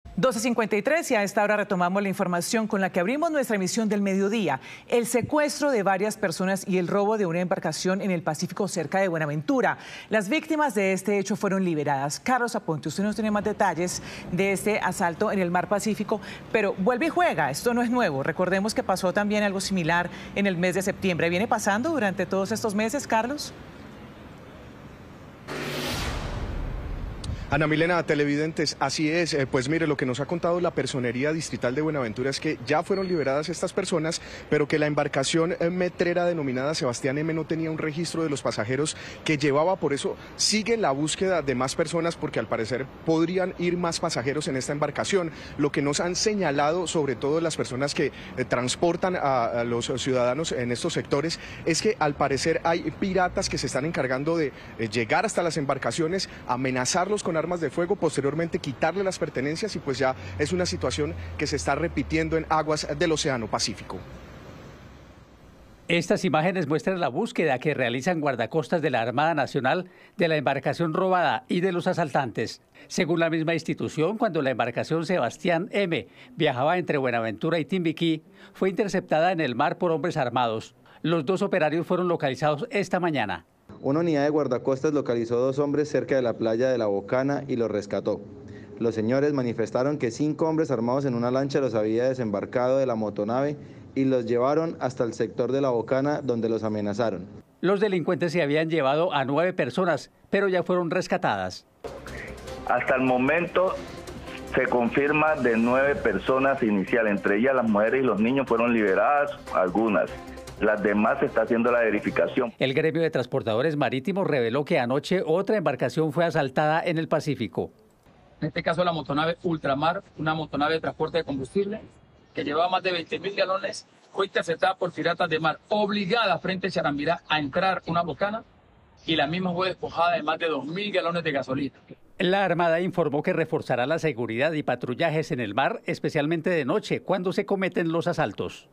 Piratas secuestraron a varias personas y robaron una embarcación en el océano Pacífico
Guardacostas de la Armada Nacional están tras la pista de una embarcación robada en el Océano Pacífico y de los asaltantes. Según la misma institución, cuando la embarcación Sebastián M viajaba entre Buenaventura y Timbiquí fue interceptada en el mar por hombres armados. Los dos operarios fueron localizados en horas de la mañana de este miércoles, 13 de diciembre.

“Una unidad de guardacostas localizó dos hombres cerca de la playa de La Bocana y los rescató. Durante la conversación, manifestaron que cinco hombres armados en una lancha los habían desembarcado de la motonave y trasladado hasta el sector de La Bocana, donde los amenazaron”, comentó el capitán de corbeta Juan David Rendón, comandante de la estación de Guardacostas de Buenaventura.